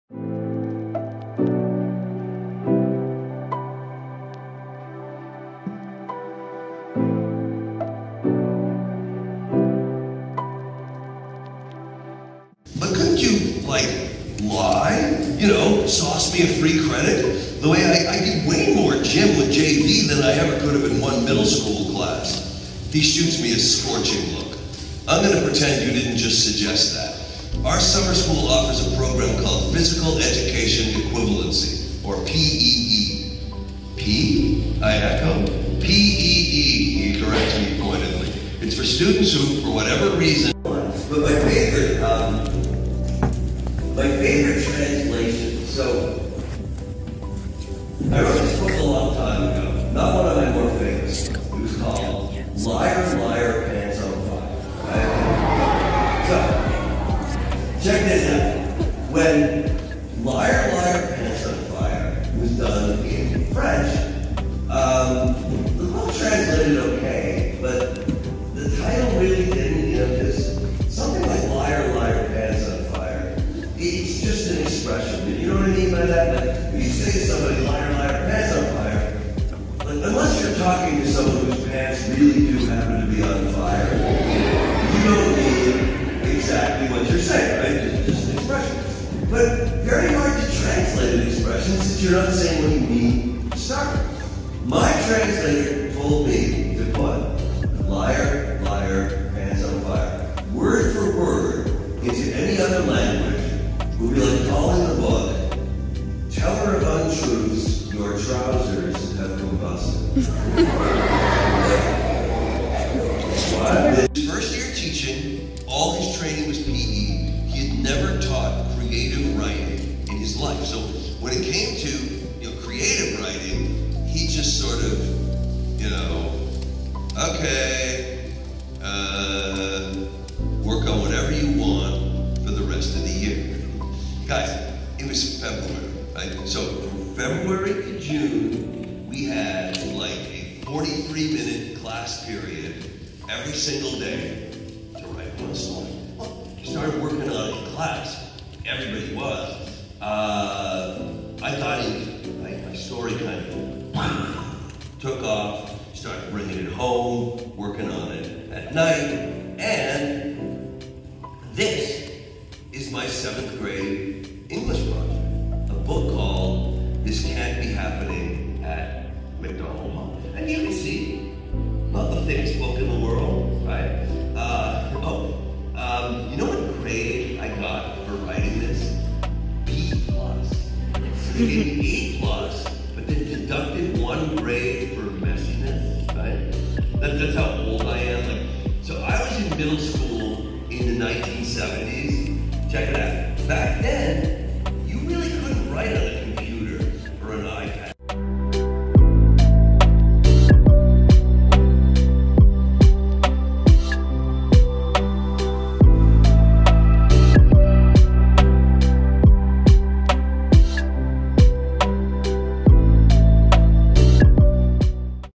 HP Author Visit 1/9/24
Author Gordon Korman met with students in grades 2-8.  Check out the highlights!